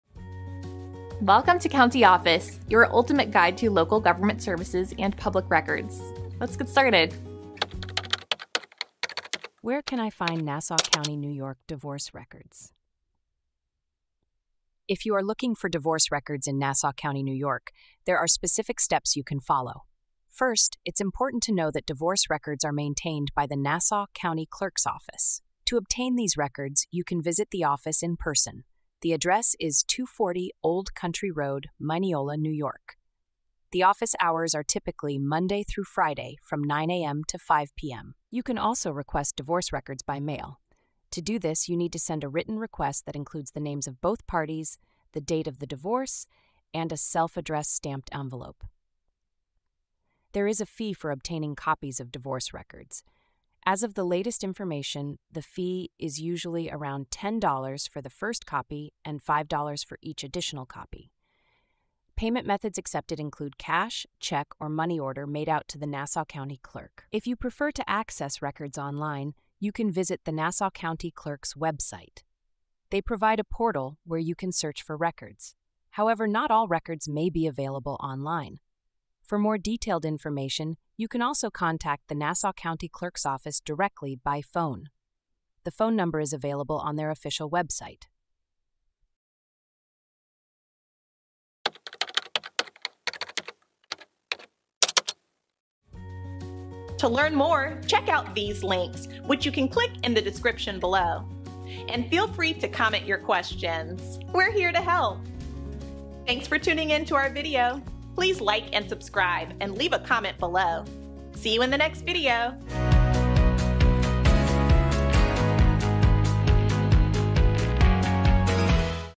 Where Can I Find Nassau County, New York Divorce Records? - CountyOffice.org
Where Can I Find Nassau County, New York Divorce Records? Have you ever needed to find divorce records in Nassau County, New York? In this video, we provide a step-by-step guide on where and how to locate these important documents. Divorce records in Nassau County are maintained by the Nassau County Clerk's Office, and we'll walk you through the various methods of obtaining them. Whether you prefer an in-person visit, mail request, or online access, we've got you covered with all the necessary details and procedures. Stay informed and empowered when it comes to accessing divorce records in Nassau County, New York.